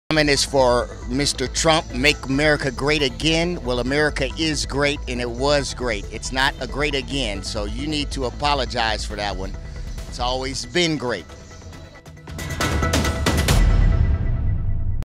Rocky De La Fuente: join the conversation 2
The President is the person elected by the people to create value for our society.

In order to do that, he or she has to understand what is important to the people and that is achieved by listening to them.